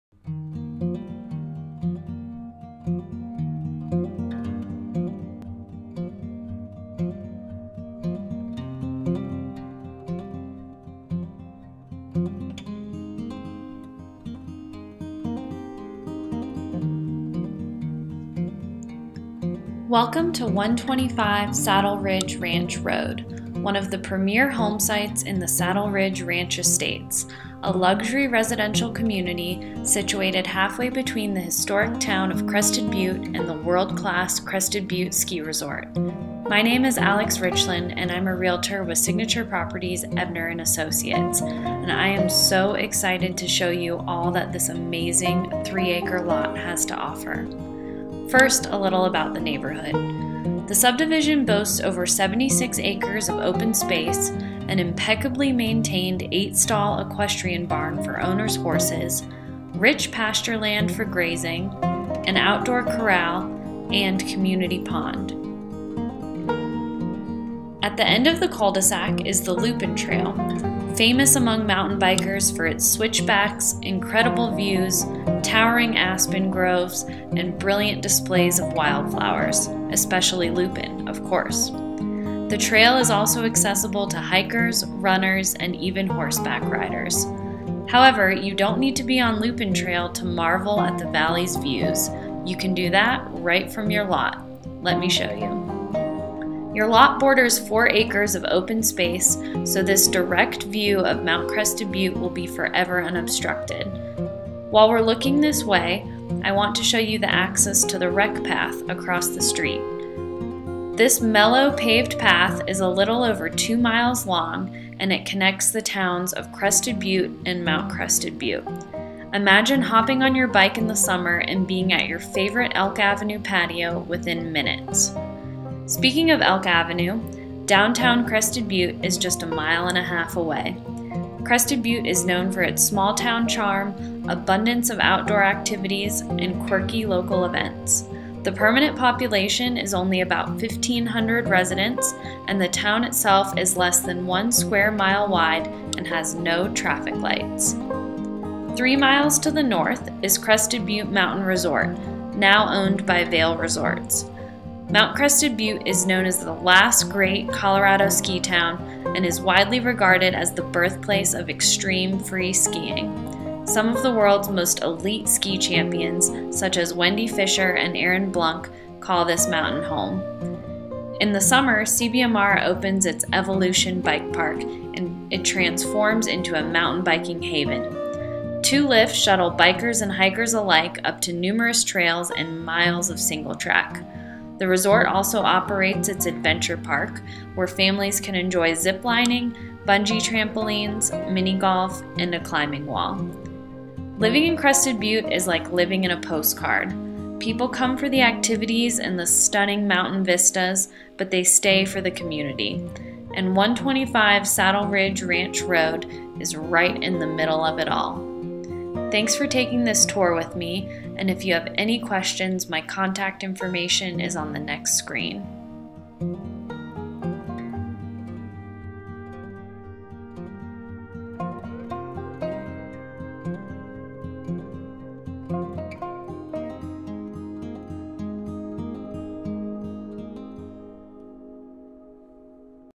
125 Saddle Ridge Ranch Road
Take a narrated Google Earth tour of this incredible 3 acre lot for sale in Crested Butte, CO! Are you ready to build your mountain dream home? Photos and listing information and can be found at: